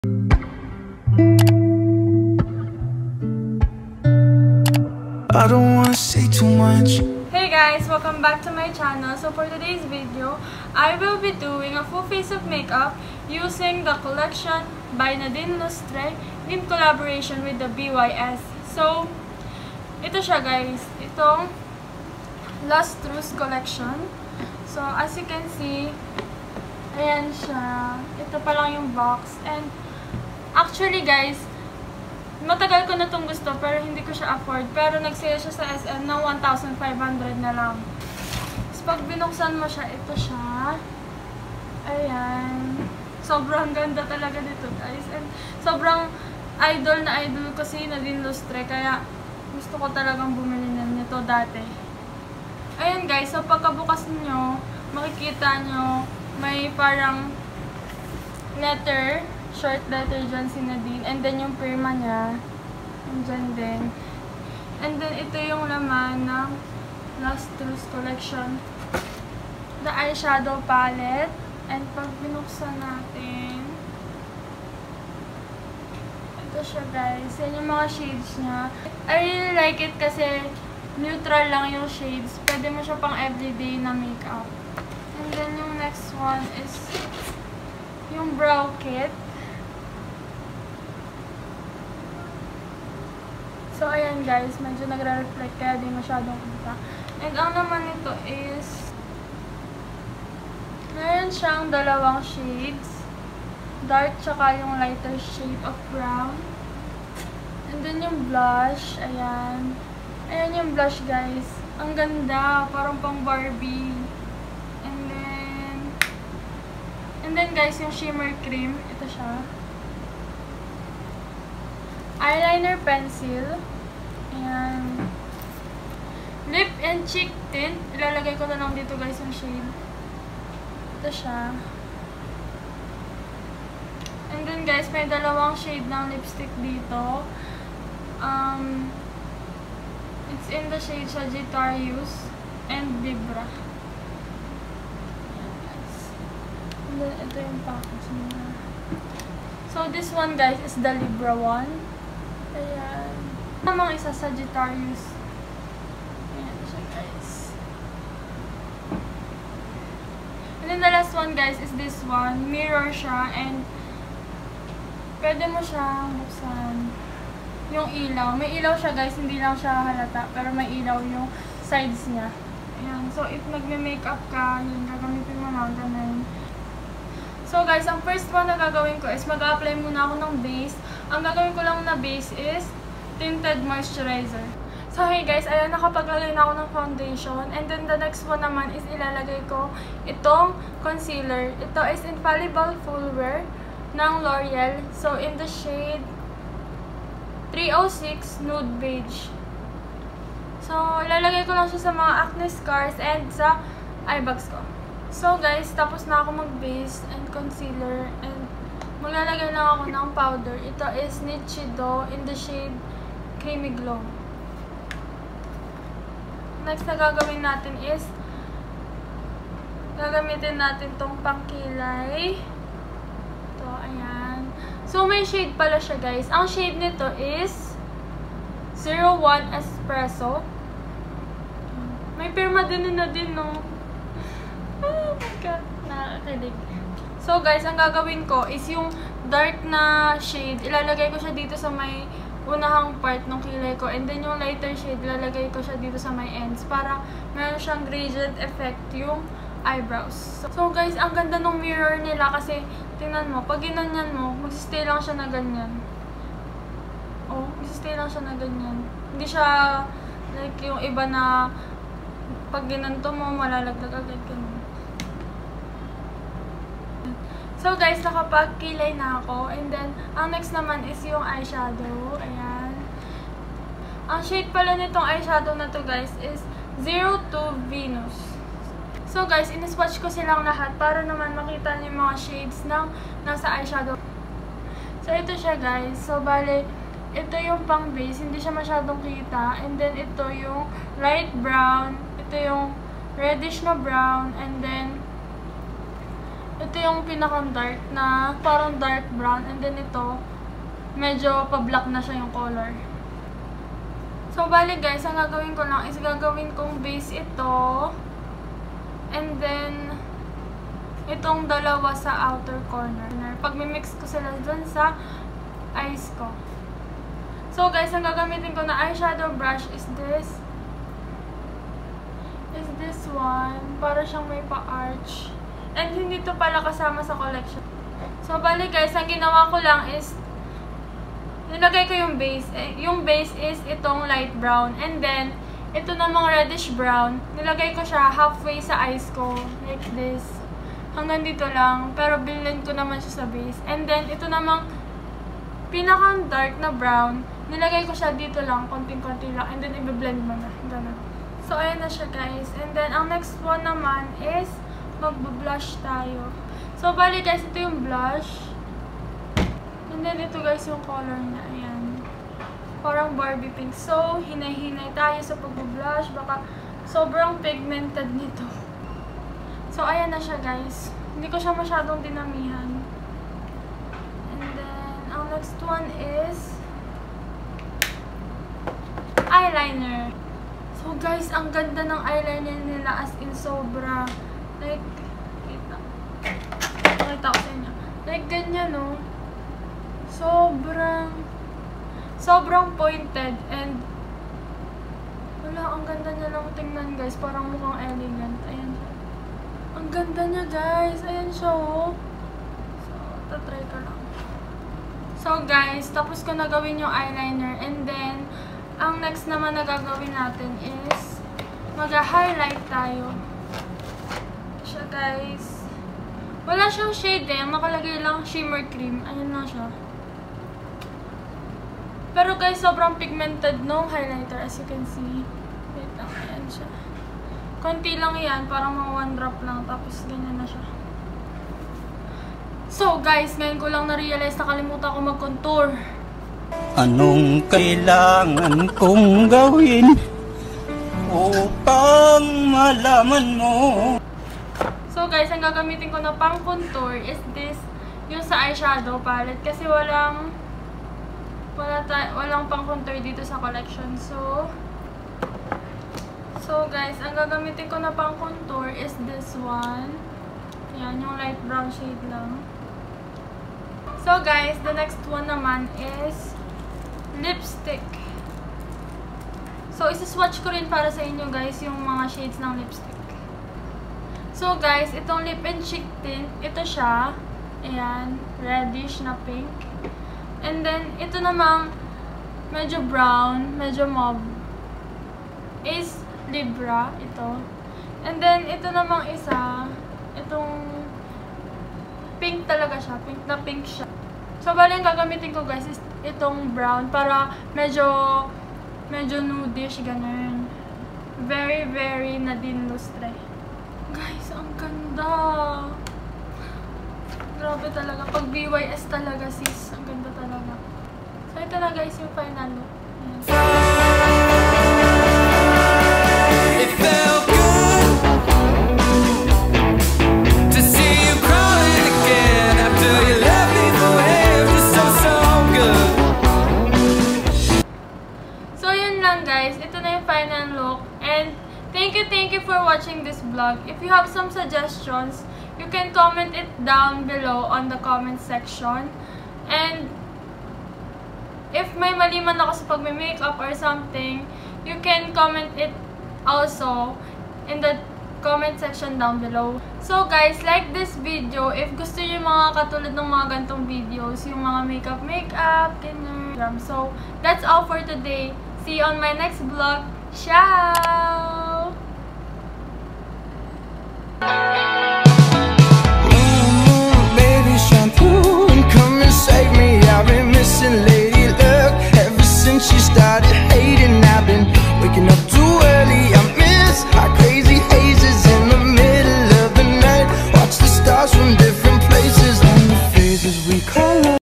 FULL FACE MAKE UP USING THE LUSTROUS COLLECTION (NADINE LUSTRE X BYS COLLABORATION)
c    i    a    o!

Love,
Harumi ♡